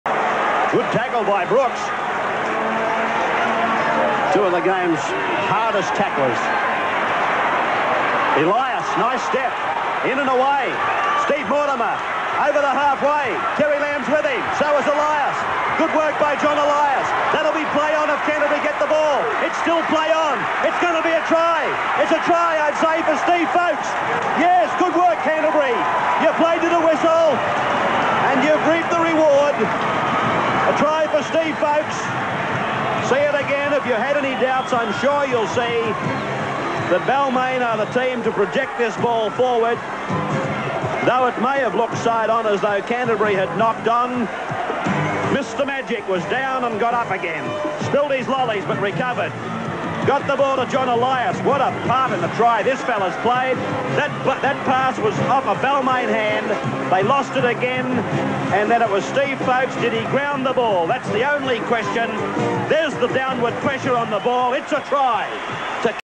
Steve Folkes Try 1986 Rd 1 Canterbury v Balmain
Steve Folkes Try 1986 Winfield Cup Round 1 Canterbury-Bankstown Bulldogs v Balmain Tigers at Leichhardt Oval.